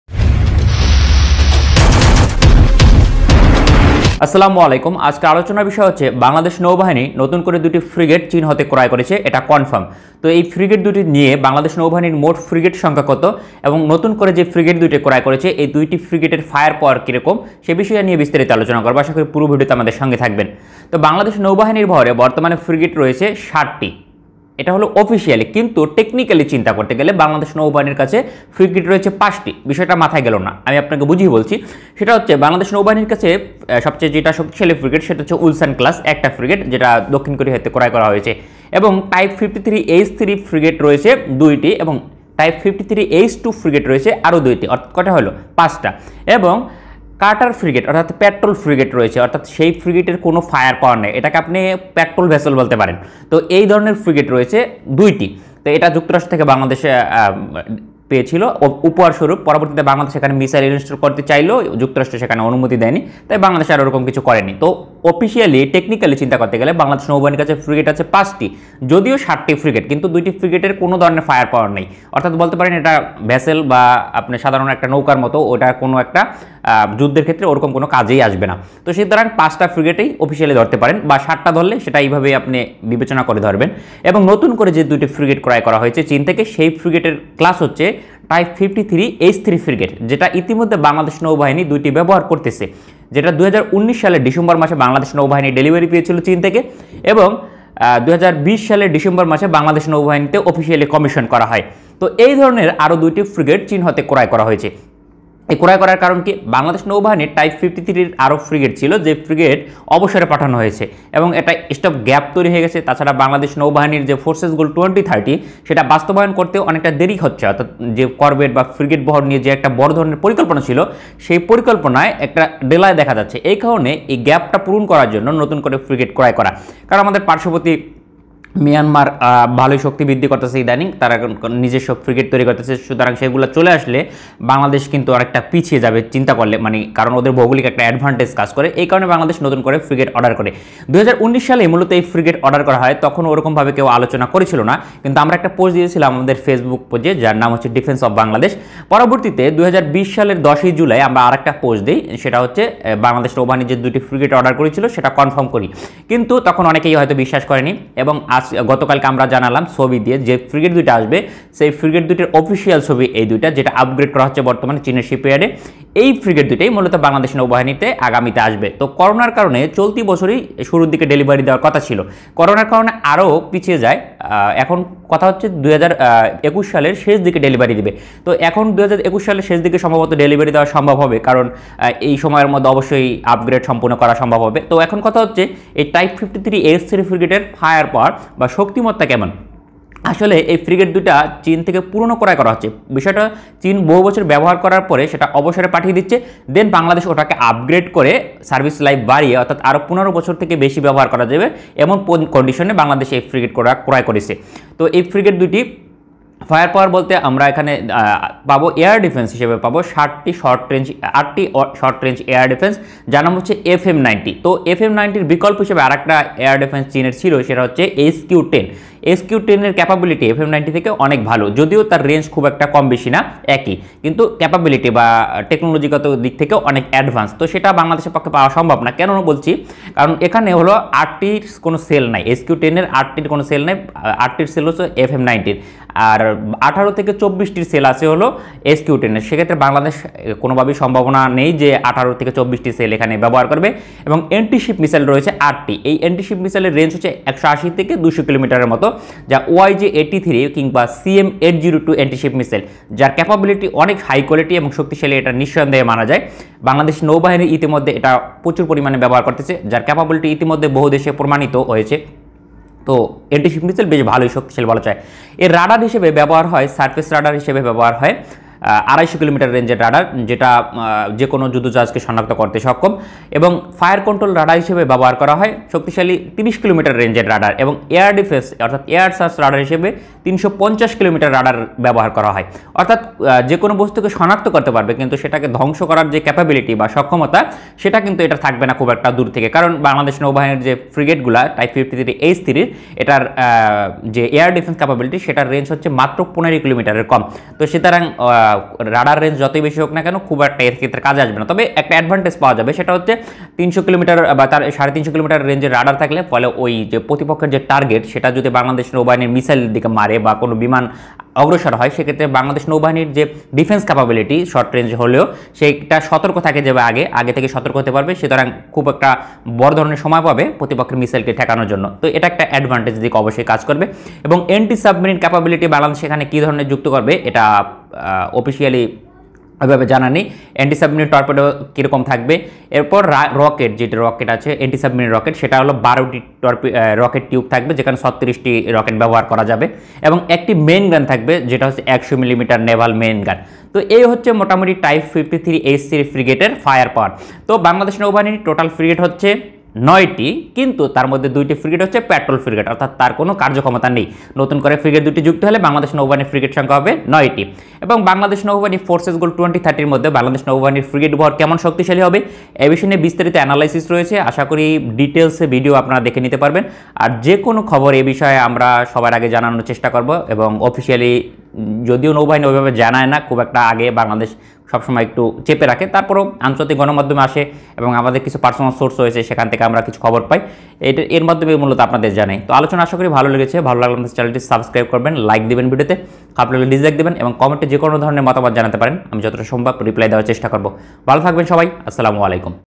Bangladesh Navy buy Two New Frigate From China Confirm |🇧🇩 যুদ্ধ জাহাজ ক্রয় করেছে বাংলাদেশ চীন থেকে
🔹Bangladesh Navy buy two Type-53H3 Frigate from China.

Defense Analysis OF Bangladesh-DOB